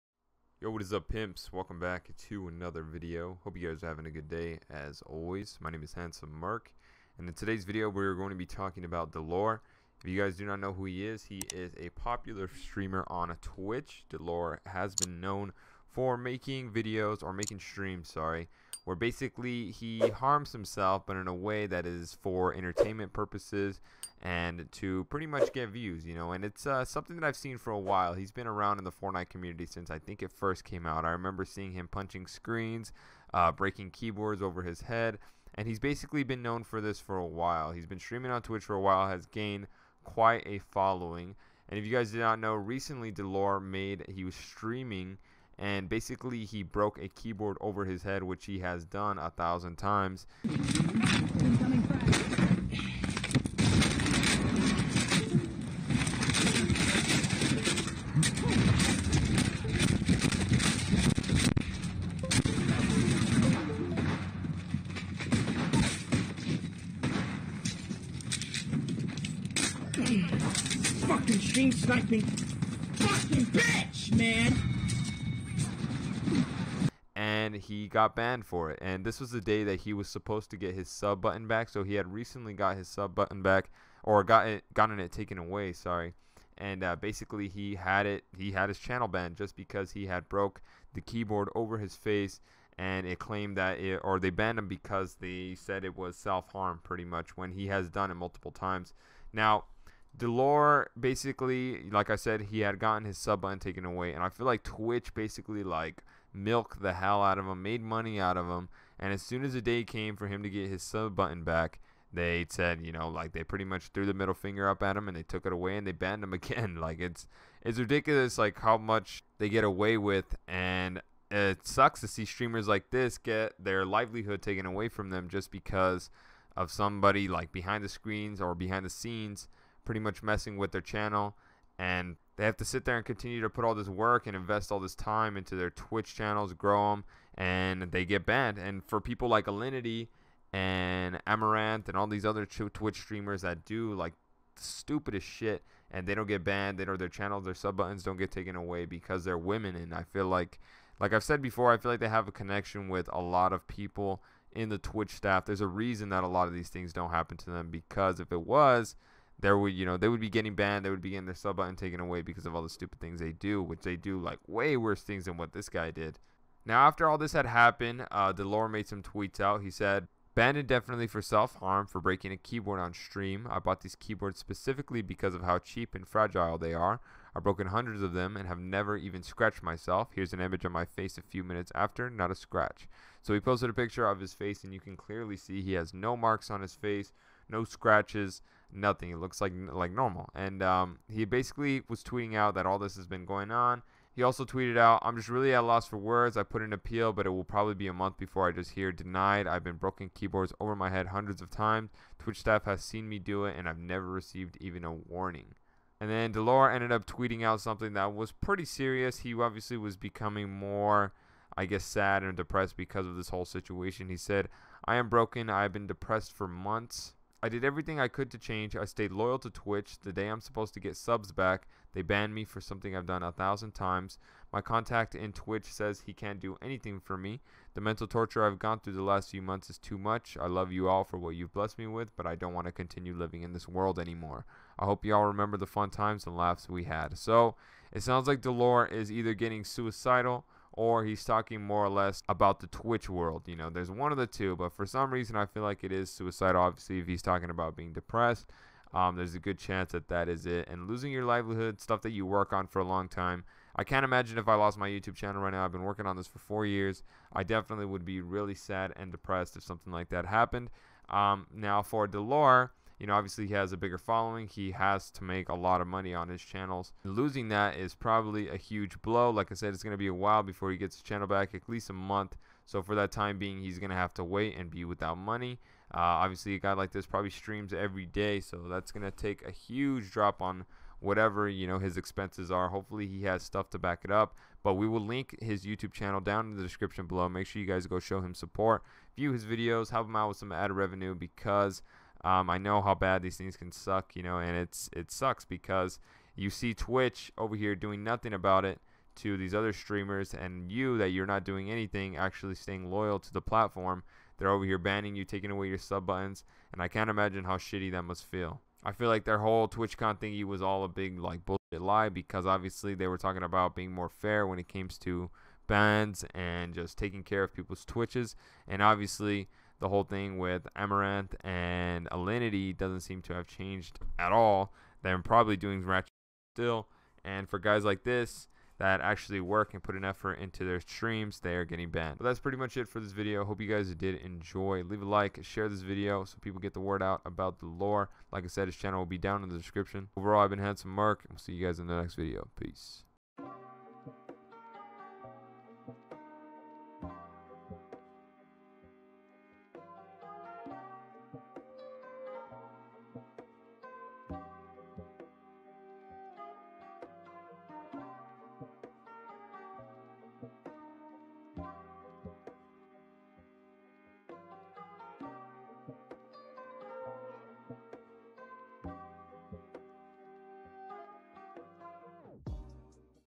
Dellor Perma Twitch Banned for "Self Harm" - Dellor Posts Dangerous Note After Ban
Dellor Perma Twitch Banned for "Self Harm" - Dellor Posts Disturbing Note After Ban